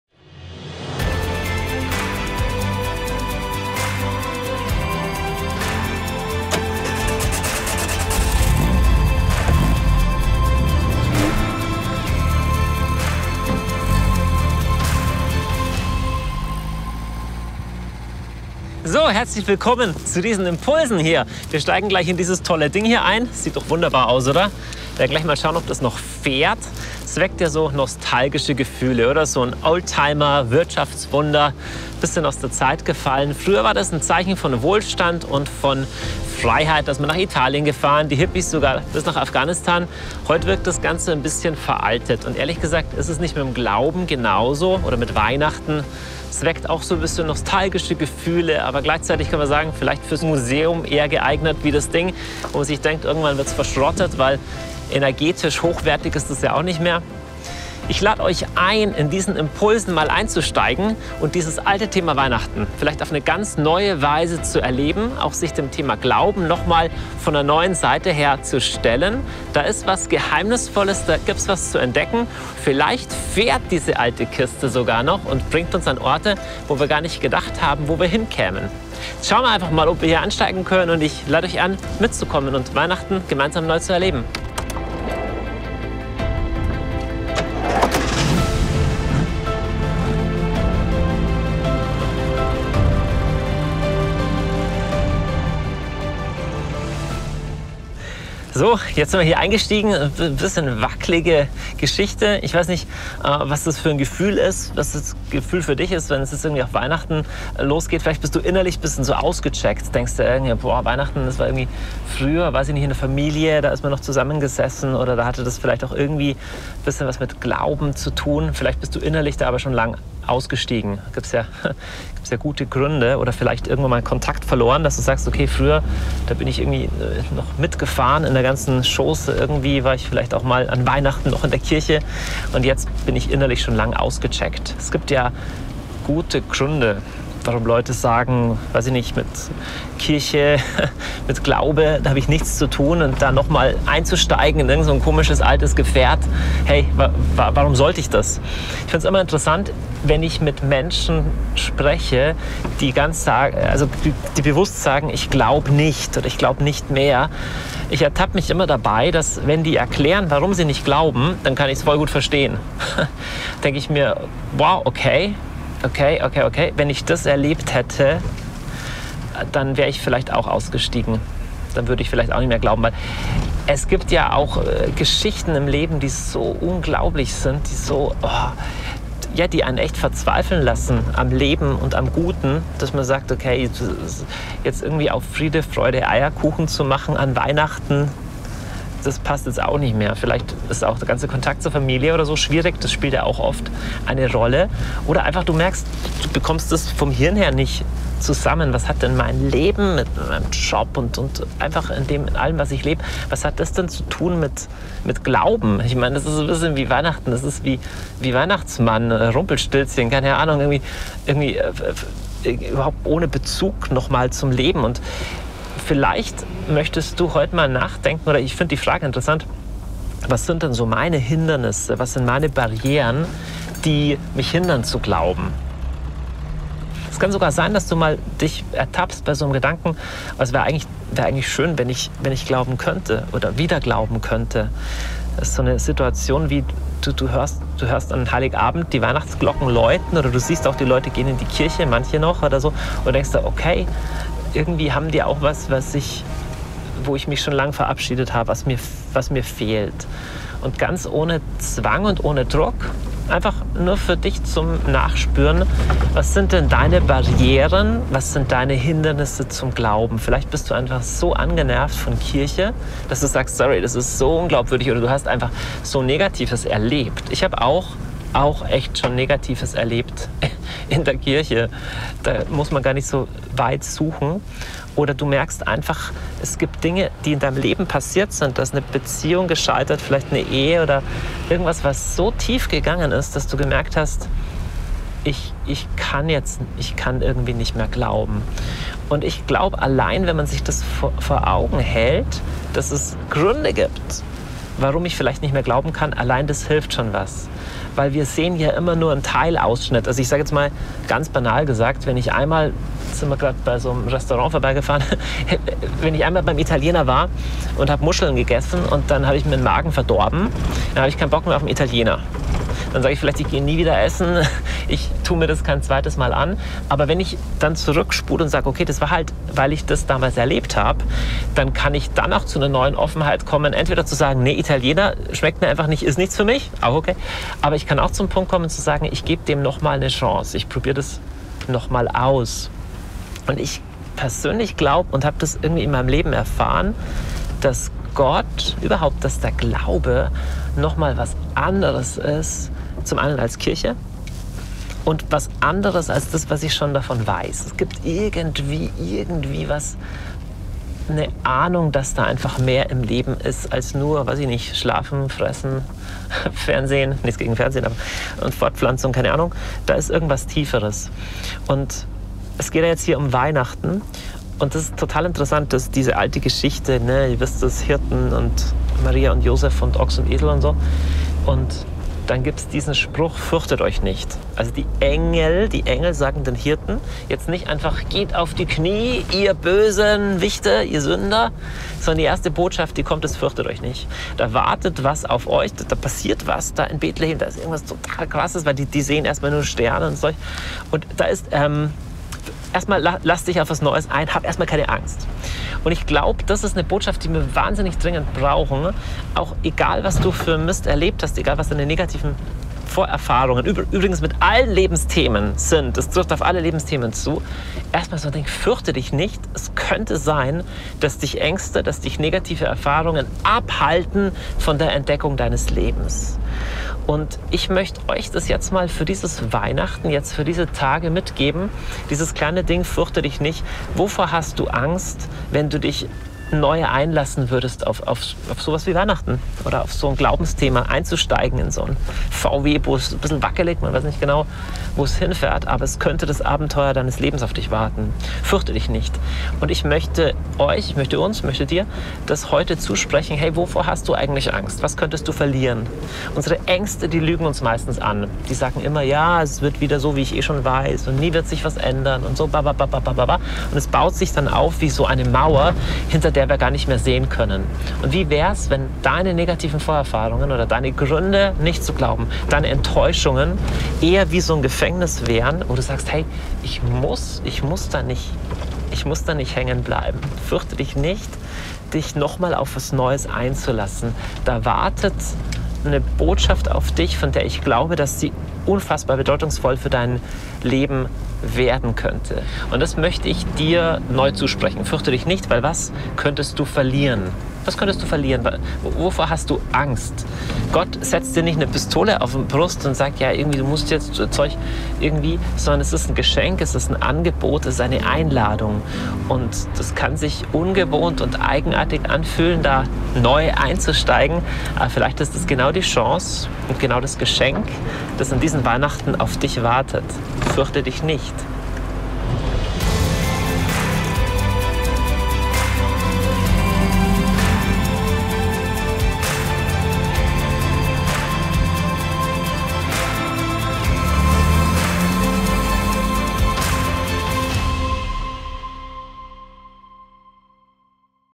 Impuls 1. Adventswoche - Glaube als Herausforderung | Dr. Johannes Hartl
Dies ist der Impuls für die erste Adventswoche mit Dr. Johannes Hartl. Das Thema ist: Glaube als Herausforderung, passend zu den ersten 6 Tagen des Buches "24x Weihnachten neu erleben“ von Oskar König.

Der nächste Videoimpuls ist ab dem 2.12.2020 online verfügbar.

Werde mit Deiner Kleingruppe Teil der Weihnachtsaktion "24x Weihnachten neu erleben". Wir unterstützen Dich mit vier Videoimpulsen von Johannes Hartl und vier Gesprächsleitfäden für Deine Kleingruppe.